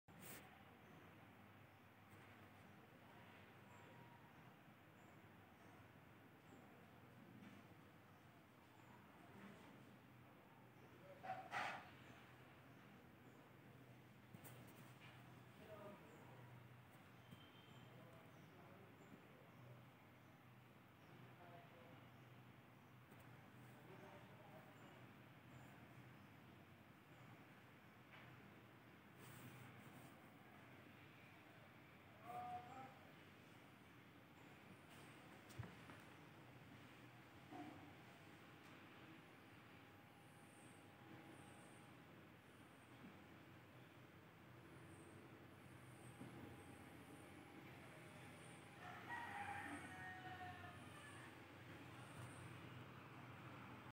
🔴Live Dear Padma Morning 01:00 p.m. 02/12/2021 Nagaland State Lottery Result Today
🔴 First Prize Last Digit Morning/Day/Evening

Morning 1:00 p.m. & Day 6 p.m. TARGET Number